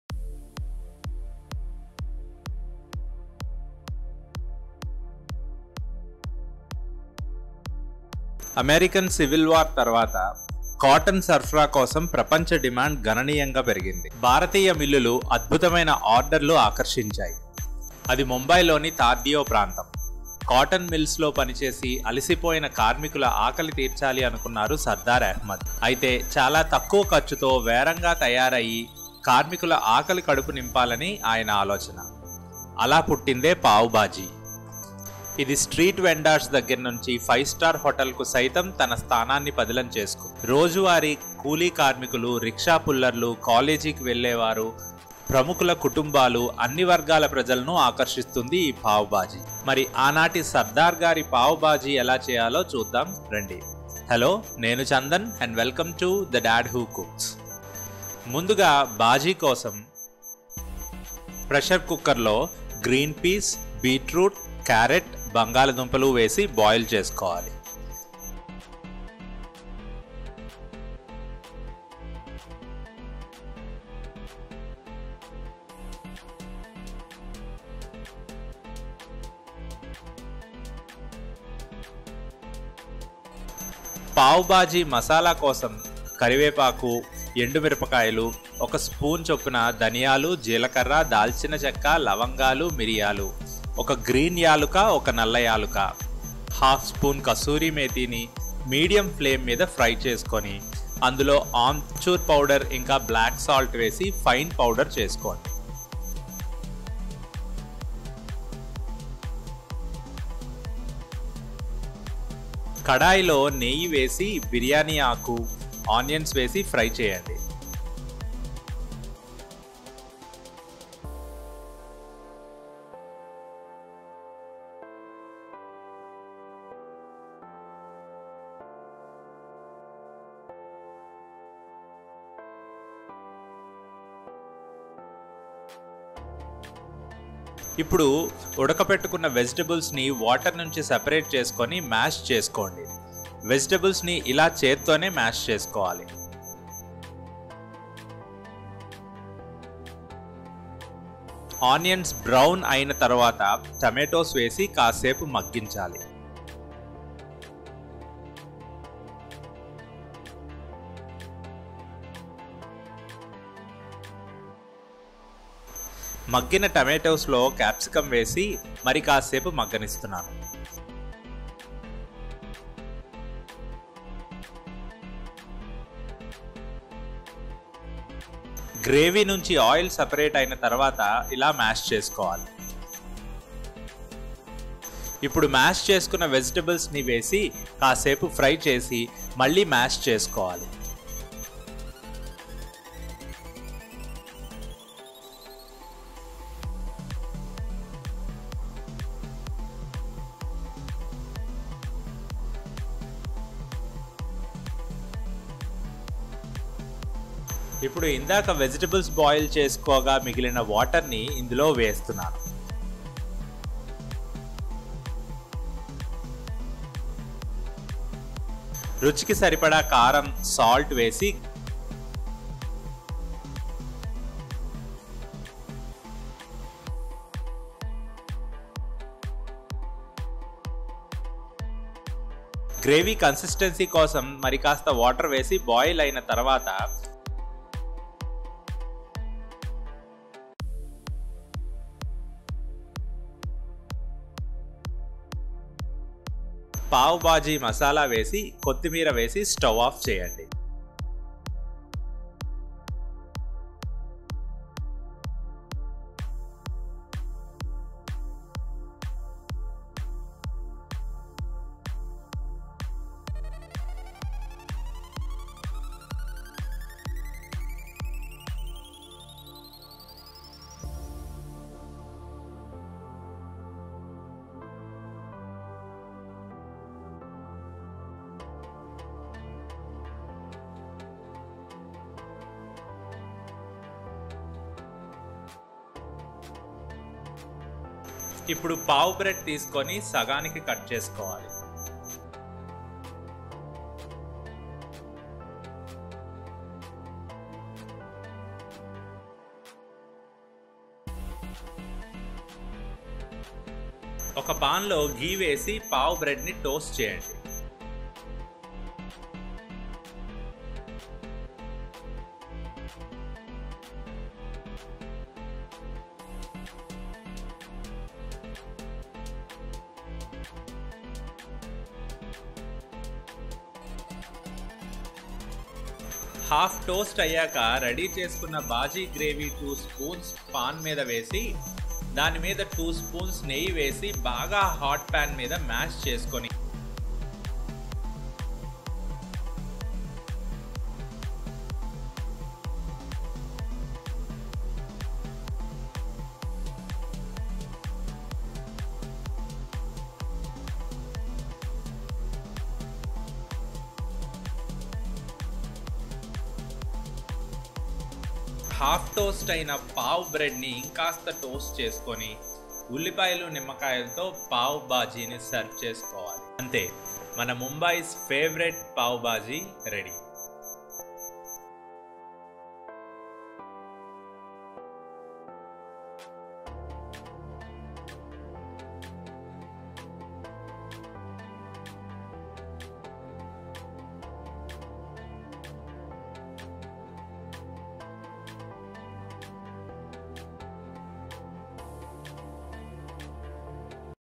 Mumbai’s Favorite Sardar’s Pav Bhaji
With the dawn of the American Civil War (from 1861 to 1865), a sharp rise in the global demand for cotton supply saw Indian mills attract gargantuan orders. Meeting them meant backbreaking work around the clock for textile mill workers in the island city of Mumbai. So much so, they hardly had time to feed themselves.
In this scenario, there was a direct need for a snack that would be quick to prepare, eat and light on the pocket too. The answer came in the form of pav bhaji by Shardar Ahmed  who stood outside the gates of these mills and sold them plate by plate for a few paise.
Essentially a thick curry made by mashing leftover vegetables, (majorly tomatoes for puree and boiled potatoes for the base), the dish soon became a crowd favourite. Surpassing the era of mills in the city, pav bhaji now finds its rightful place on restaurant menus
Ingredients
Pav Bread
Clarified Butter/butter
Potatoes
Peas
Beetroot
Carrots
Capsicum
Onions
Lemon
Chilli powder
Salt
Bay leaf
For Pav Bhaji masala
Cinamon
Cumin seeds
Black Salt
Black and green cardamom
Dried Mango powder
Red CHillies
Pepper
Cloves